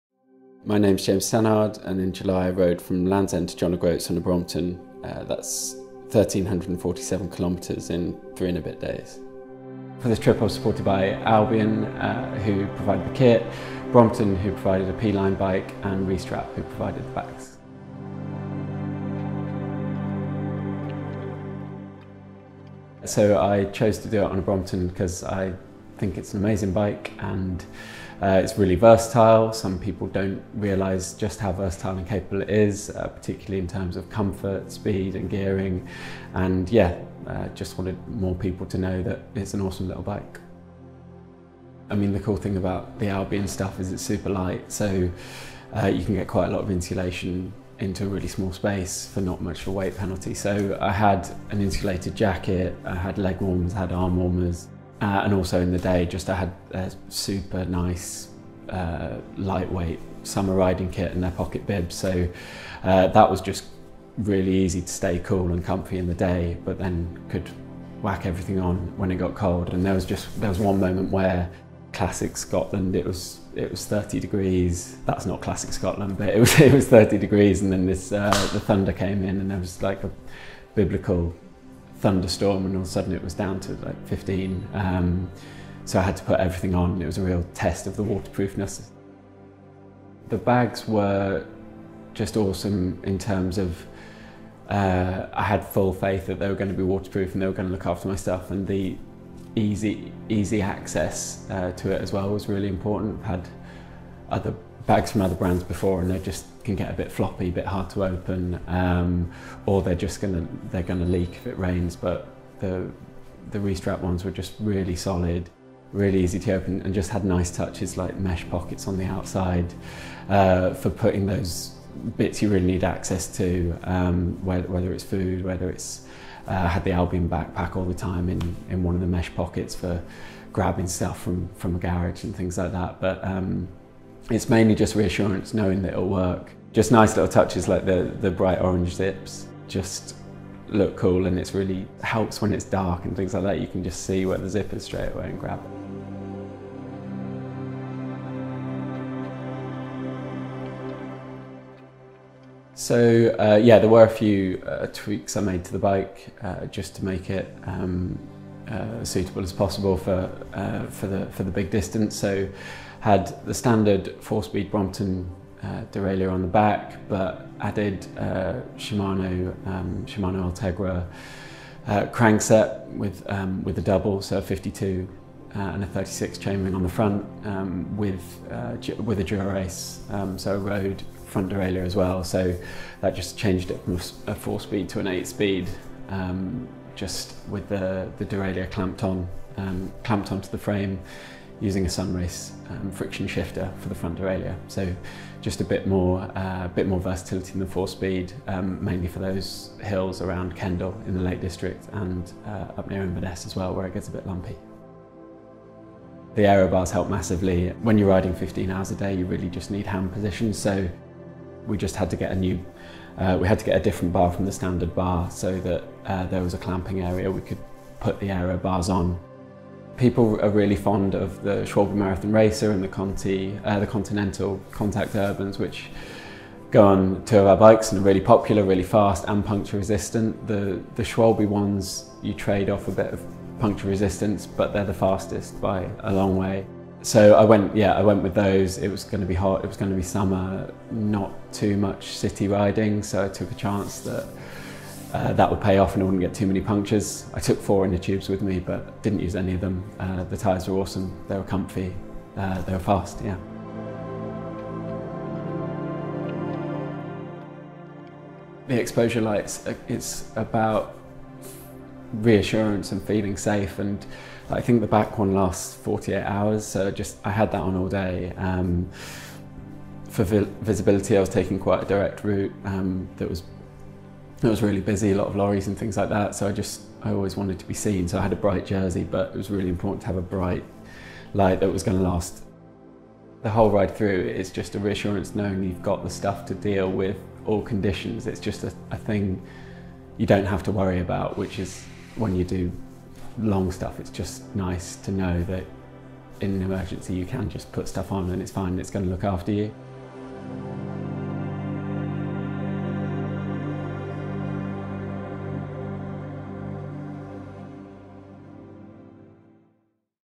Brompton, Albion and Restrap
Sigo desde hace tiempo los productos de la marca Albion, su ropa y sus bolsas para todo tipo de aficionados al mundo del ciclismo. Precisamente en su web encontré este interesante video en el que colaboran con Brompton y con la marca Restrap.
Si eres un rodador interesado en largas rutas en bicicleta, nada como la historia que nos enseña el protagonista. Los complementos perfectos para lanzarse a la aventura….